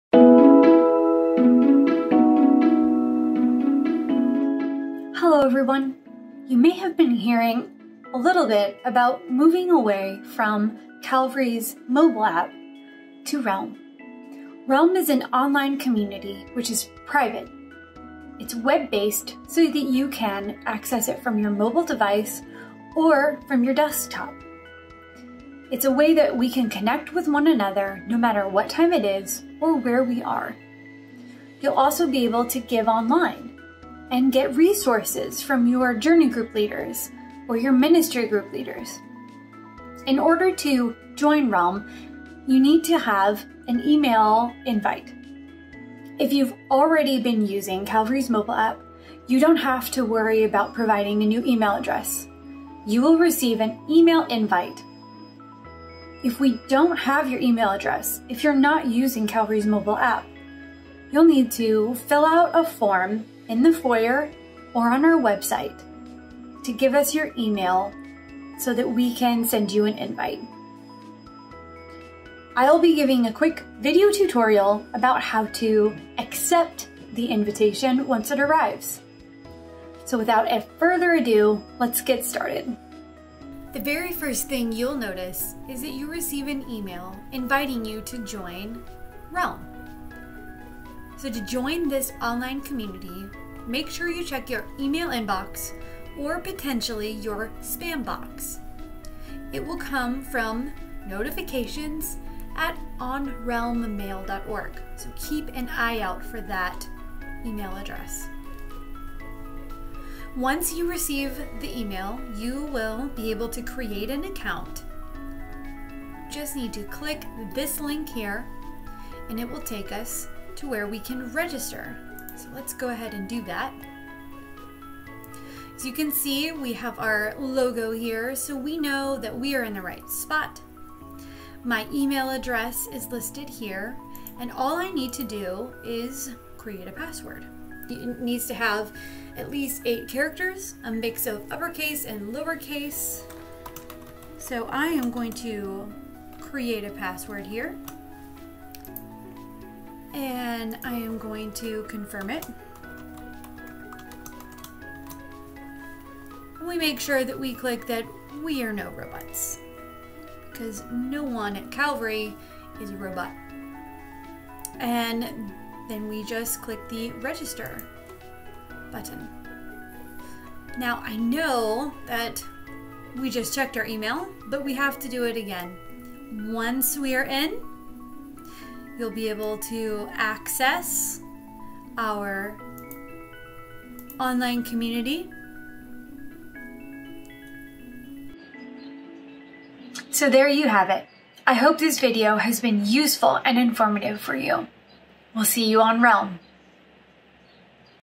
Realm Introduction & Tutorial
Learn a little bit about what Realm is and how to access it with this quick tutorial.